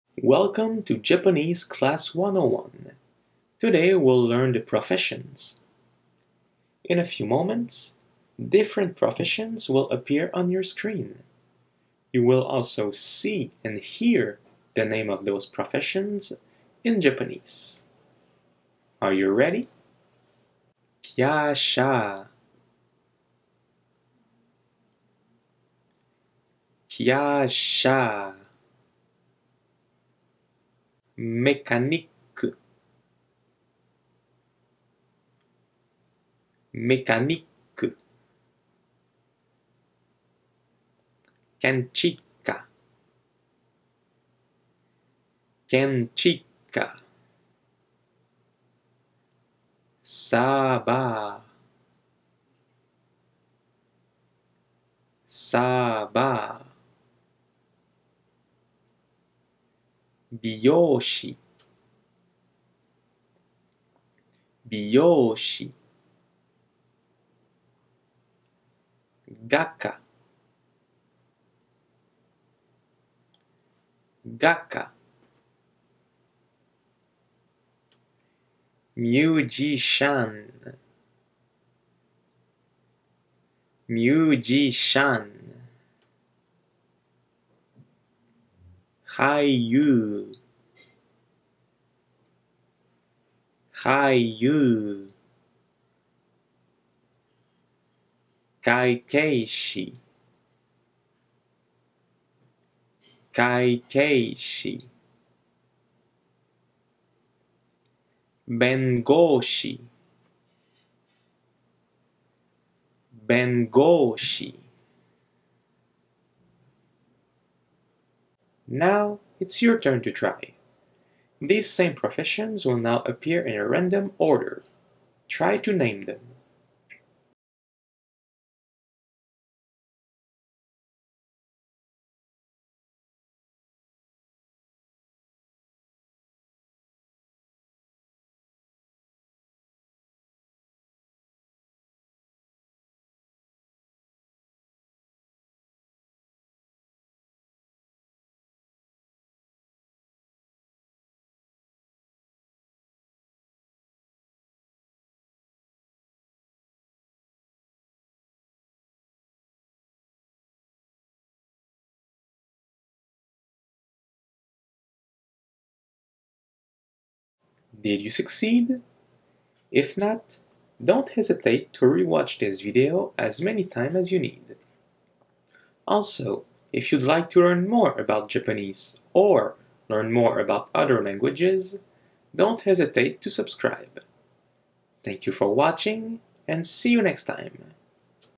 Japanese Class 101: Jobs (Part Two)
Learn the name of different jobs in Japanese